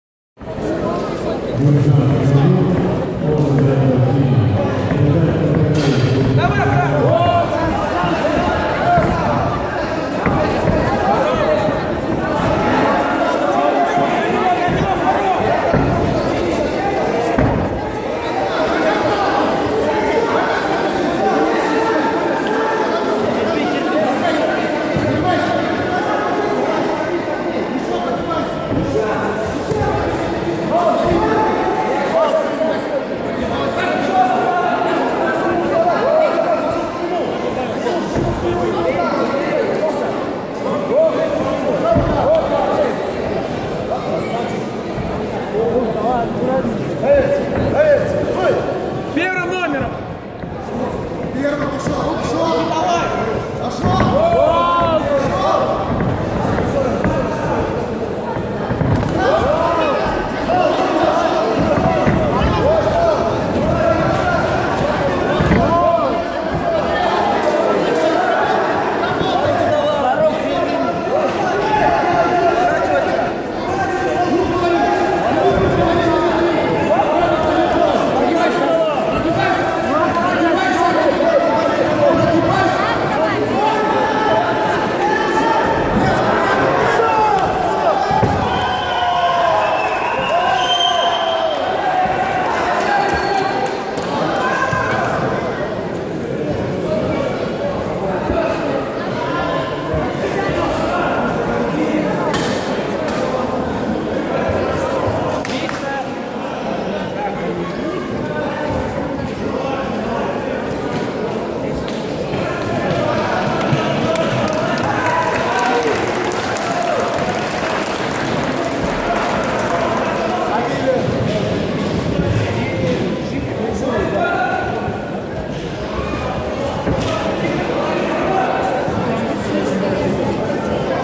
Шахты - "Кубок гладиаторов" Бои ММА - 12 (4K Ultra HD Video)
Шахты - "Кубок гладиаторов" Бои ММА - 12 (4K Ultra HD Video). «Кубок гладиаторов» по смешанным боевым единоборствам, посвященный Дню защитника Отечества. # ММА Калмыкия